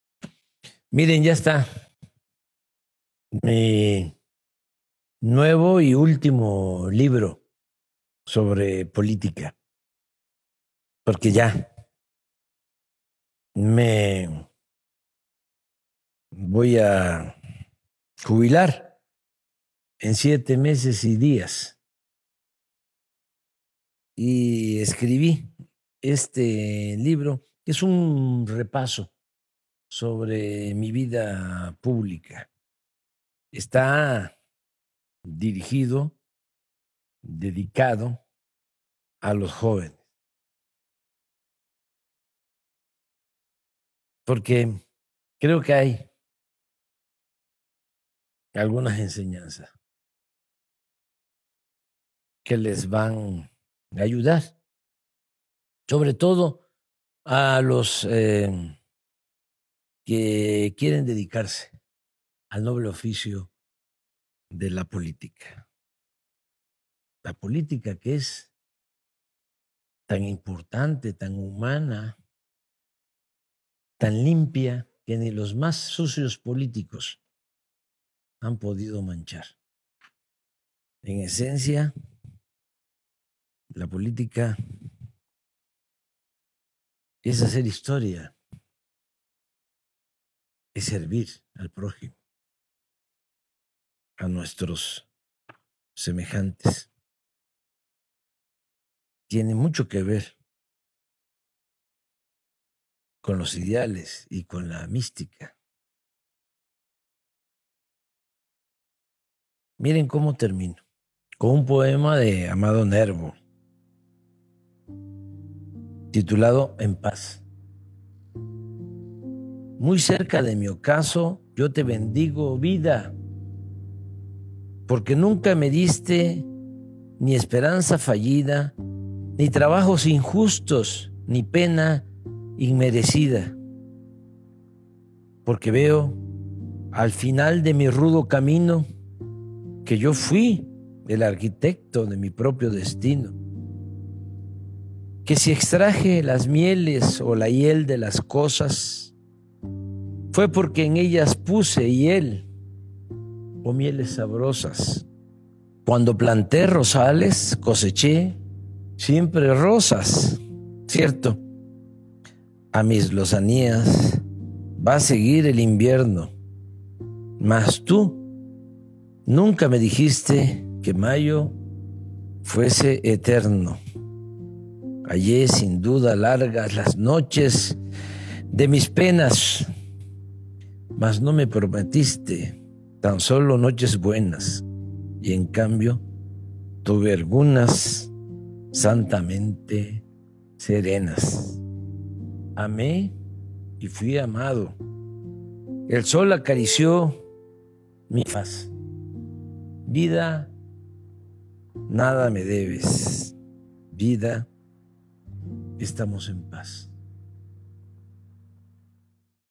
No hay que dejar de hablarle a los jóvenes. A ellas y ellos está dedicado este último libro,
No hay que dejar de hablarle a los jóvenes. A ellas y ellos está dedicado este último libro, '¡Gracias!'